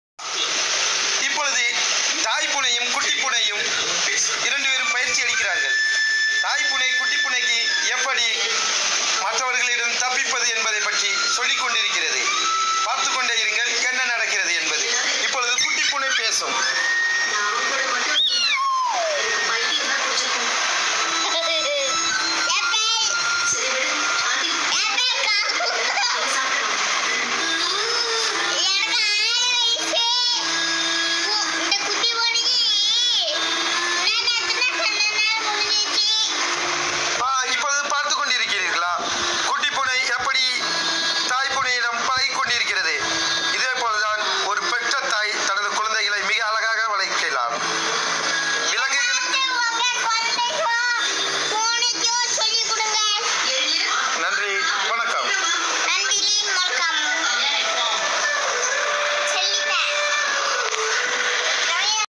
Tiger fight to cat training life is enjoying Mother cat children 🐈
எங்கள் கிராமங்களில் ஒவ்வொரு வளர்ப்புப் பிராணிகளும் எங்கள் சந்தோஷங்களை தரும் நாங்கள் மகிழ்ச்சியாக இருப்பது போல் அதுவும் எங்கள் வீடுகளில் மகிழ்ச்சியாக துள்ளிக் குதிக்கும். வாழ்க்கை வாழுங்கள்.

 Every pet in our villages will give us our happiness and just as we are happy it will jump happily into our homes.  Live life